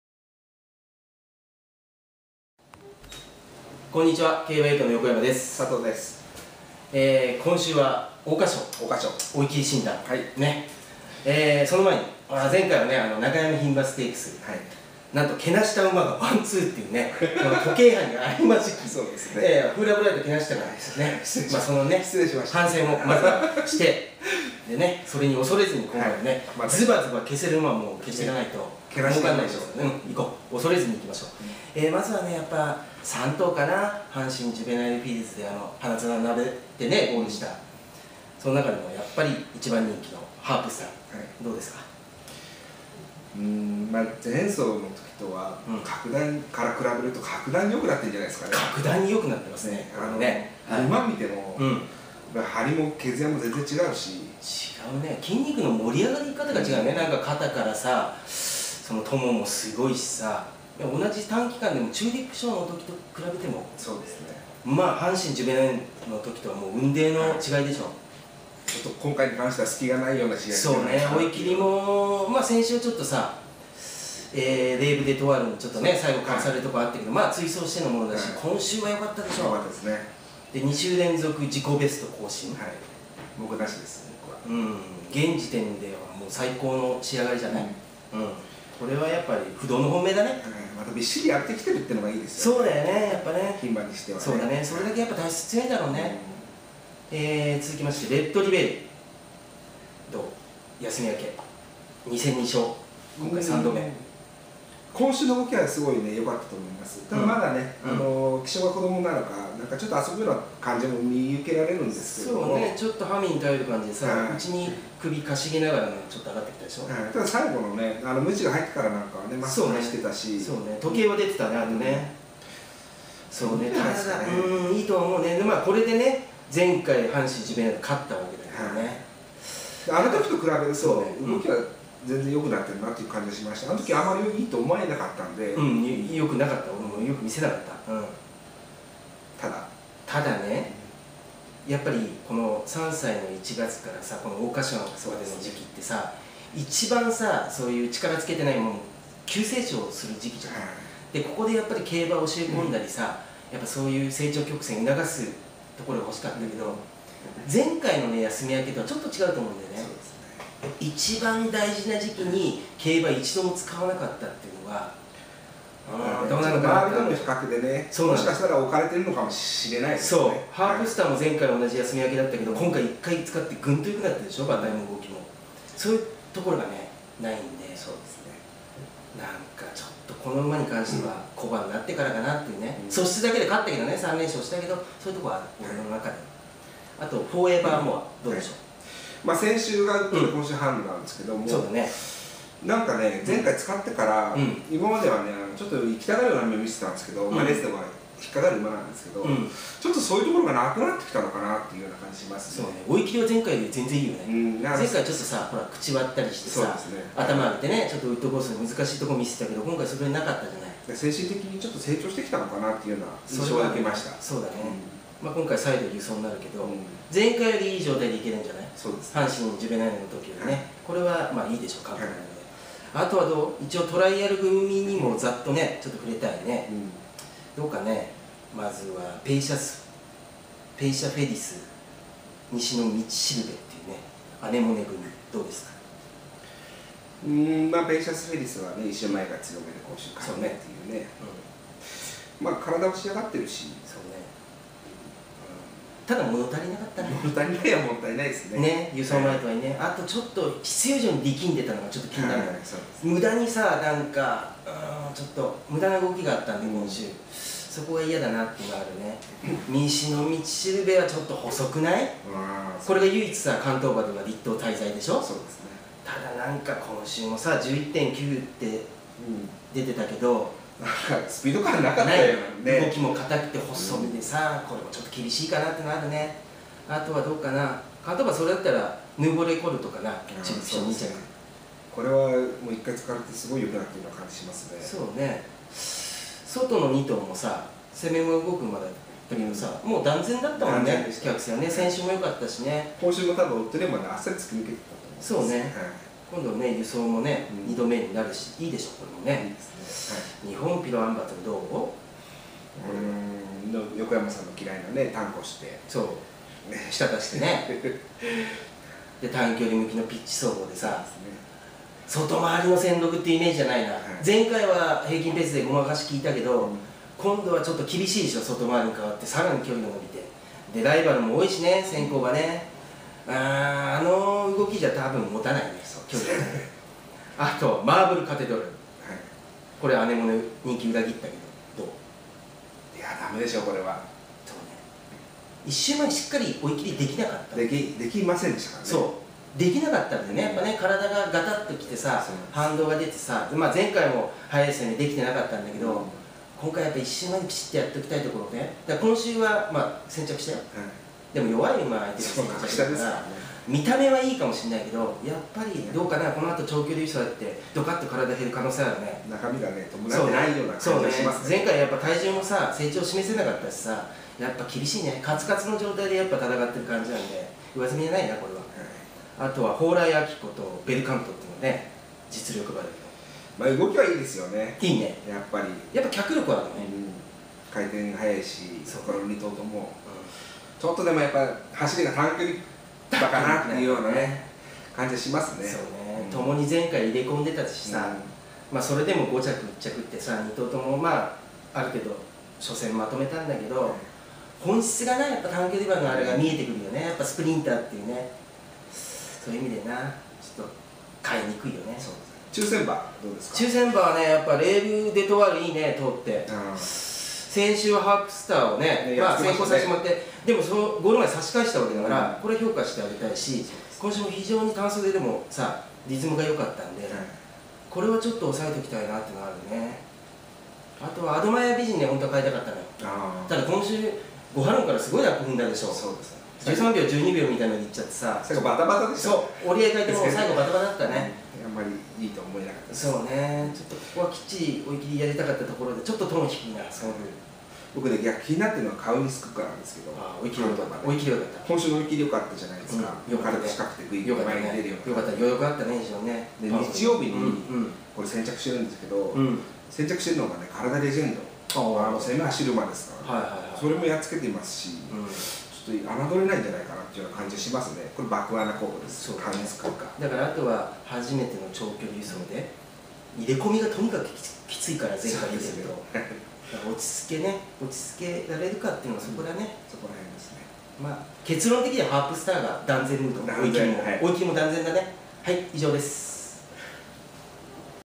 ＴＭ追い切り診断 2014桜花賞（佐藤圭＆横山）
あのコンビが桜花賞の追い切りを斬ります！　「前回は、けなした馬がワンツー」と冒頭はお詫びから始まりますが、すぐに「恐れずにいきましょう！」と横山英二ＴＭ。そう来なくっちゃ！　後半には佐藤圭ＴＭの「爆穴候補」が聞けちゃいます。馬券の参考にどうぞ。